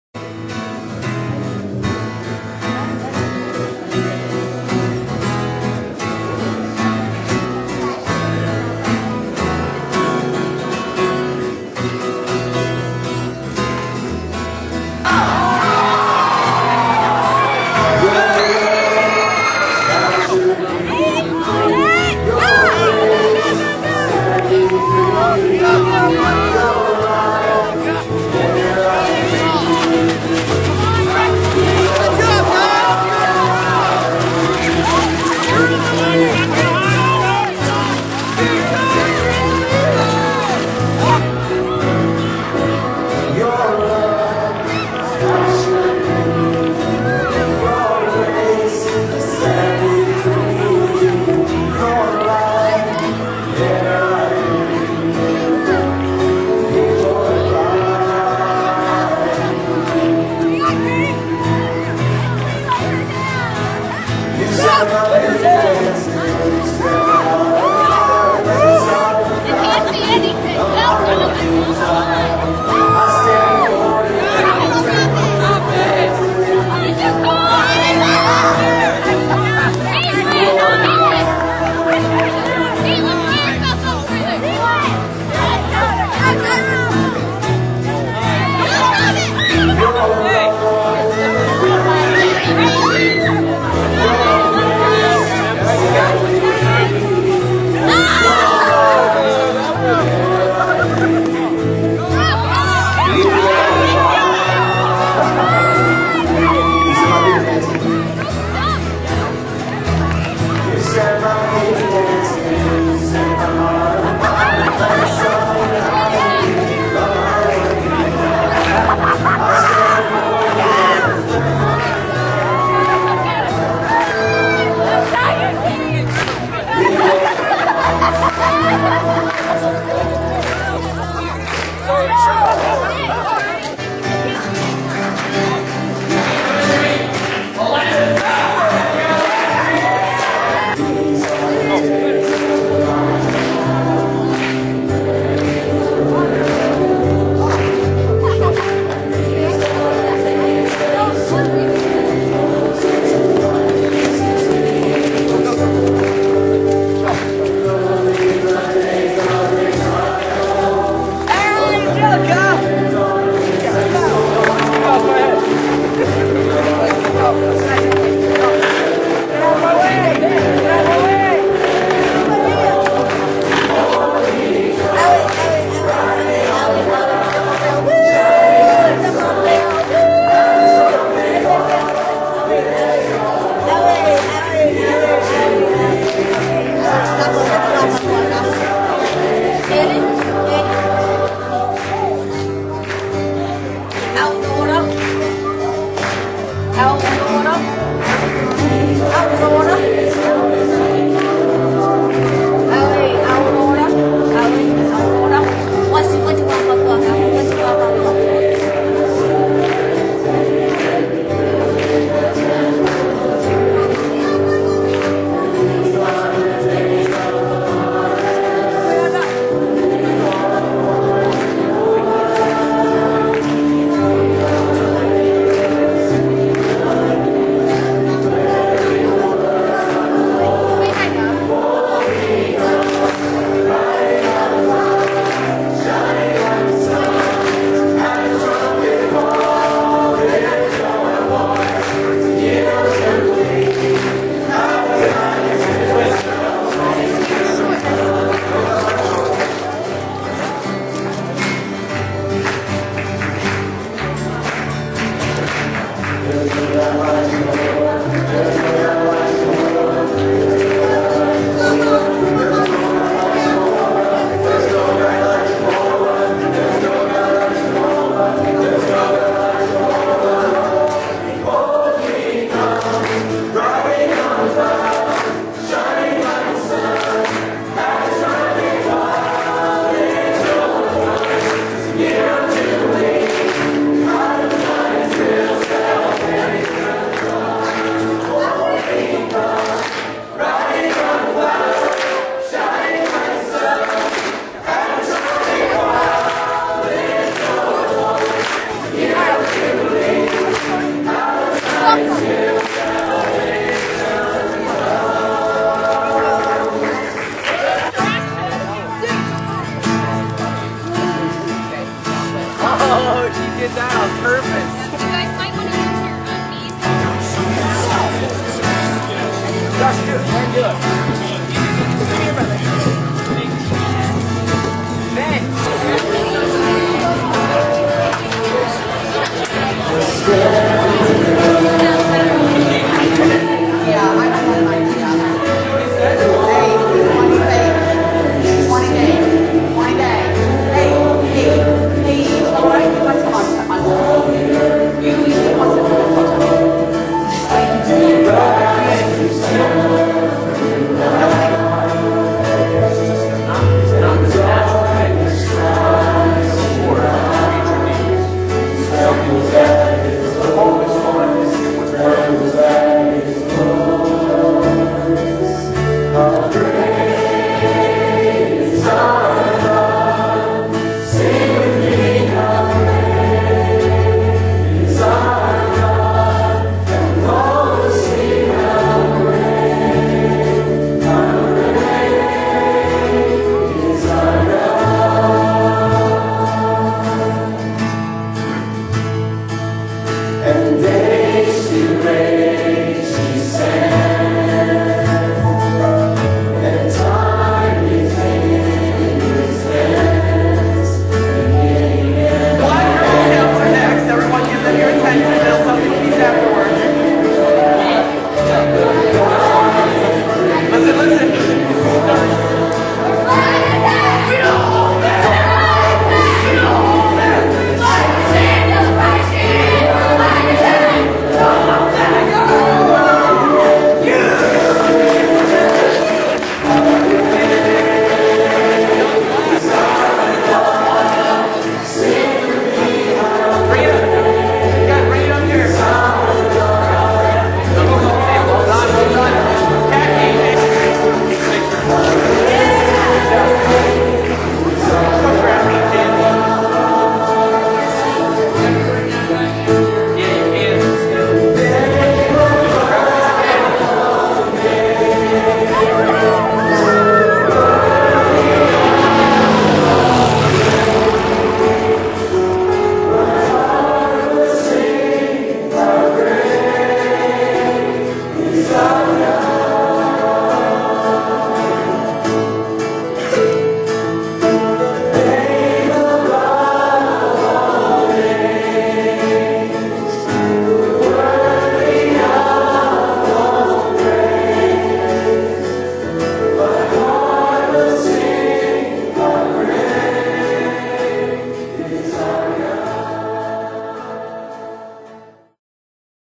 New Tribes Youth Day 2008
This is a bunch of short scenes from the recent youth day last weekend at New Tribes Missions in Jersey Shore, PA.